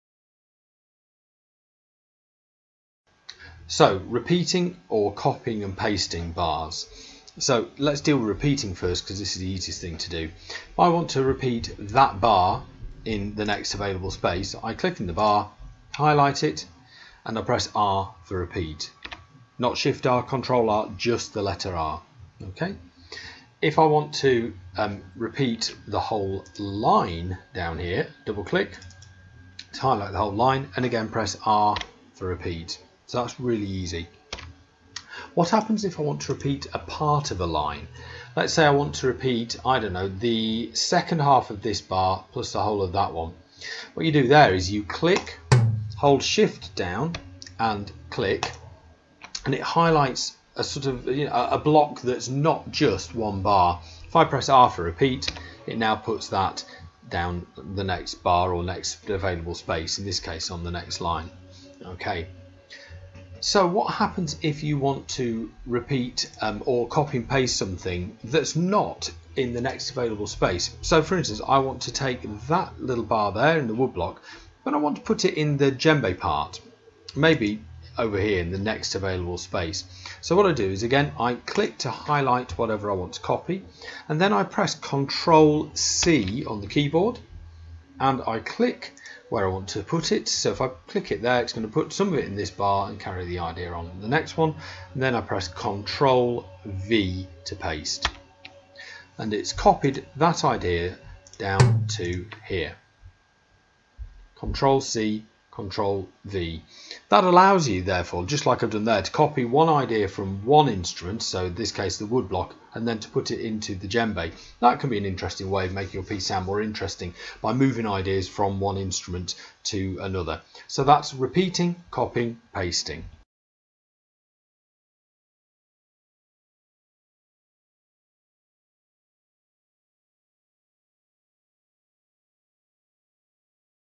Sibelius Shorts - Repeating/Copying and Pasting
How to repeat music and copy and paste music in Sibelius.